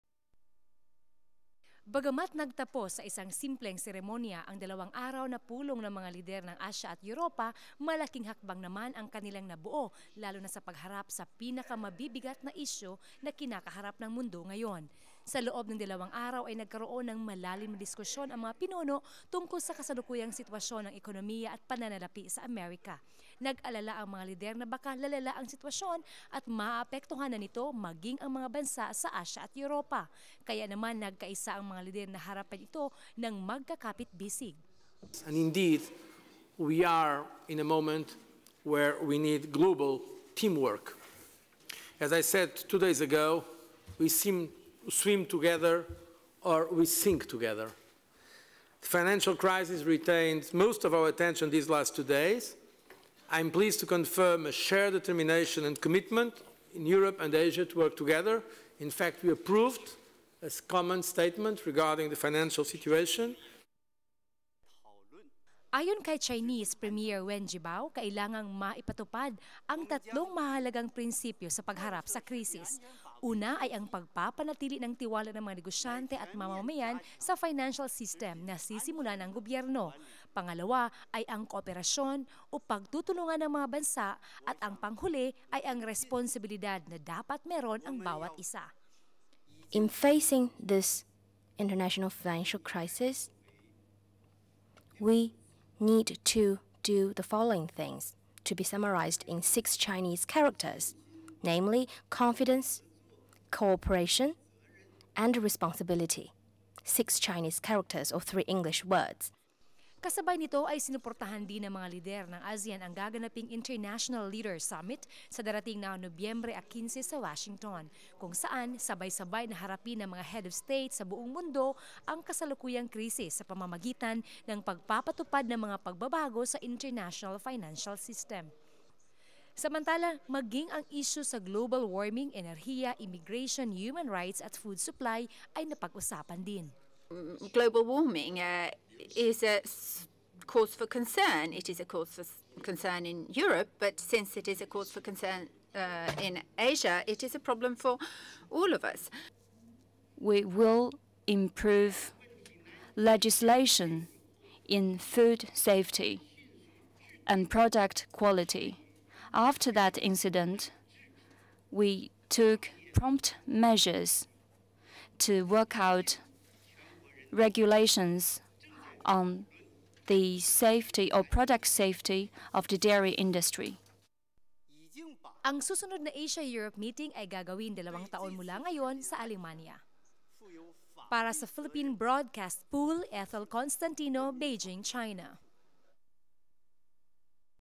CLOSING SESSION OF THE 7TH ASEM SUMMIT
The summit ended with the leaders agreeing to make full use of ASEM and other cooperation mechanisms to enhance information sharing, policy exchange, and pragmatic cooperation on supervision and management in the financial sector and effectively monitor, prevent and respond to financial risks to ensure sustained, stable and sound economic growth. President Gloria Macapagal Arroyo also echoed the reminders of other leaders that the fundamentals of free trade and specific challenges facing developing nations must be properly recognize.Great Hall of the People, Beijing, China.